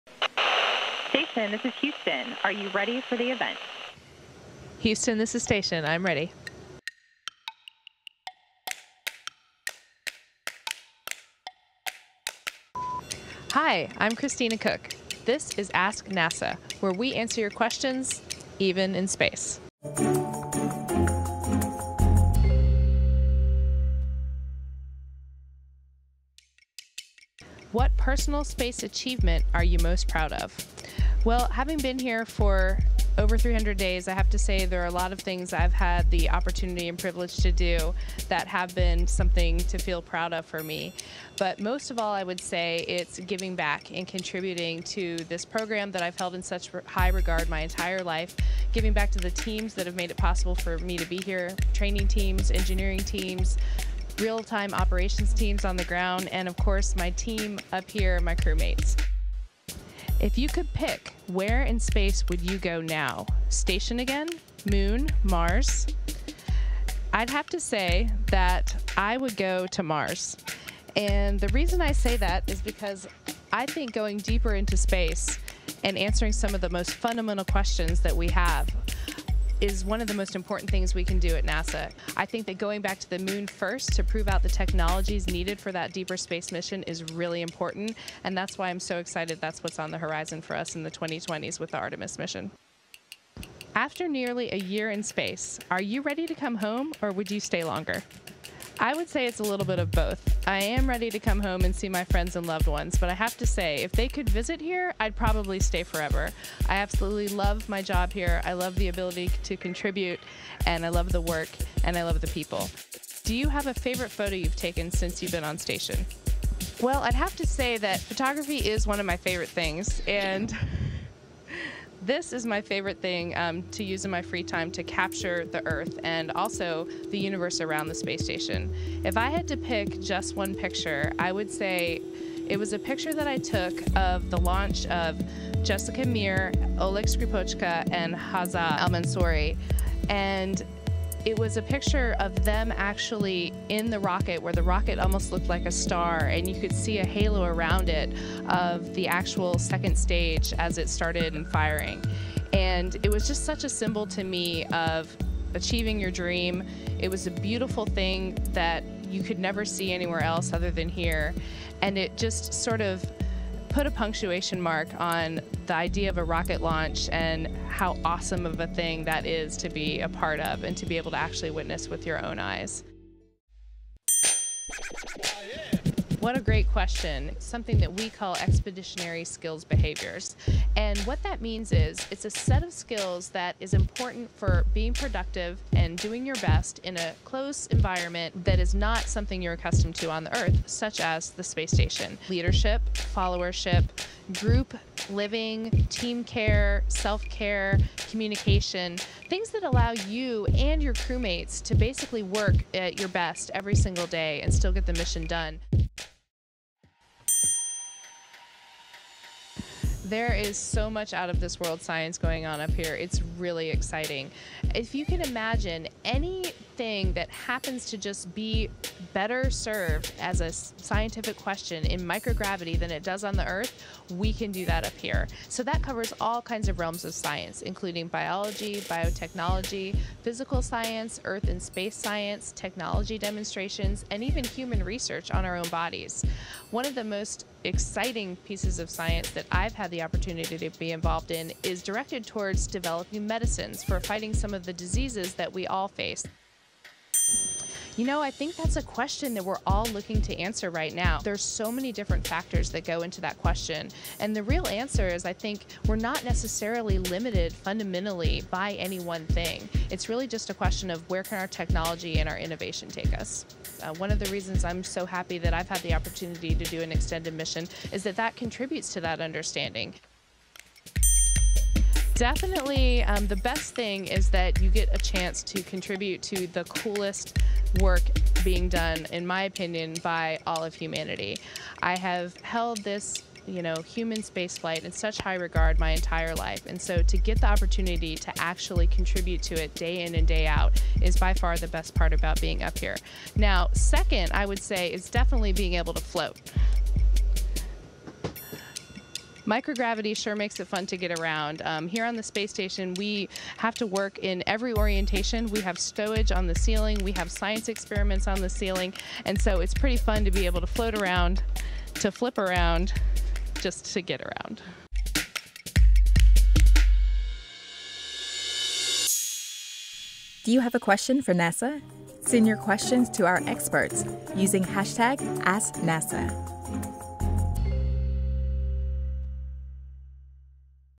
AskNASA with Christina Koch┃ What’s it like to live in space for a year?
A special “out of this world” episode of AskNASA.

During her record-setting mission NASA astronaut Christina Koch answers key questions about her stay in space. Highlighting her contributions and scientific achievements while aboard the International Space Station.

Including her help to develop medicine in microgravity and collect data needed for longer missions.

Conducting the longest single spaceflight by any woman, Christina gives great insight into questions related to future missions to the Moon with the Artemis missions and future exploration of Mars.